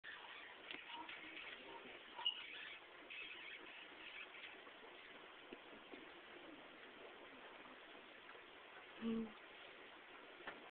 Fight!!!!!!
About a 10 sec video of Hamsters fighting.